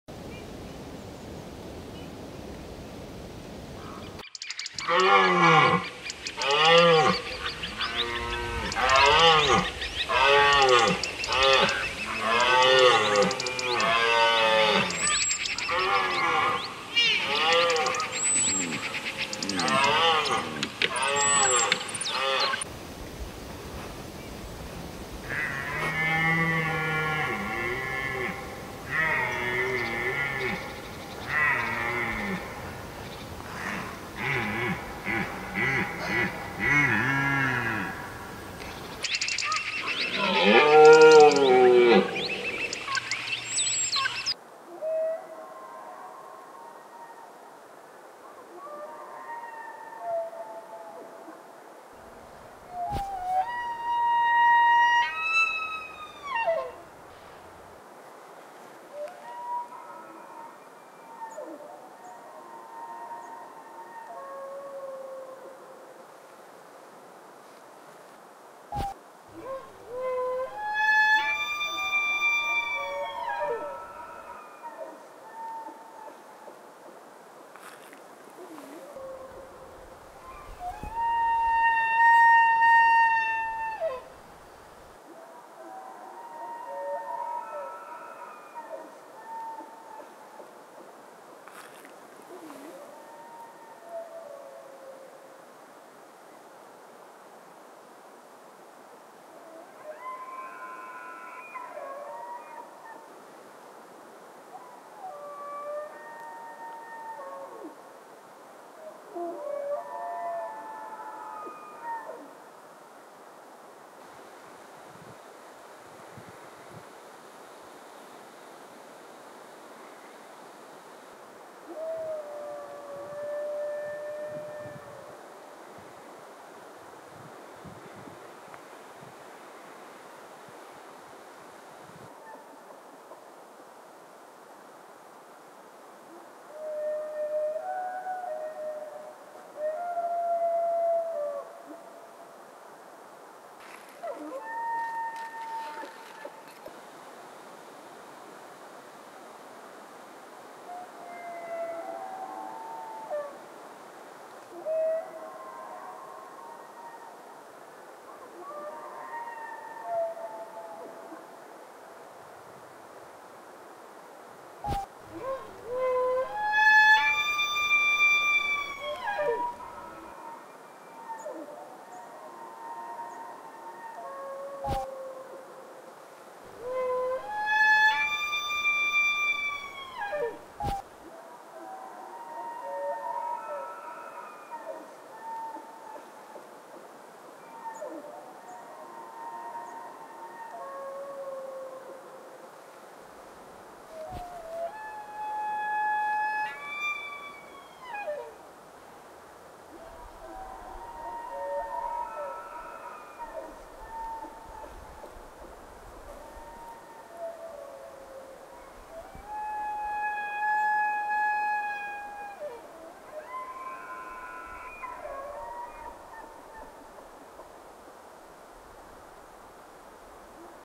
Рёв европейского оленя и марала - почувствуйте разницу! | Film Studio Aves
Сибирский марал (00:44-03:41) и европейский олень (00:00-00:44) - подвиды благородного оленя. Насколько они отличаются по голосу и внешним признакам - судите сами.
Россия. Алтайский биосферный заповедник. Озеро Телецкое.
Беларусь. Березинский биосферный заповедник, ТК "Красный Бор".
Видео - Игорь, Илья Бышнёвы (AVES).
Спасибо сотрудникам Алтайского заповедника! И отдельное спасибо - Юрию Николаевичу Калинкину!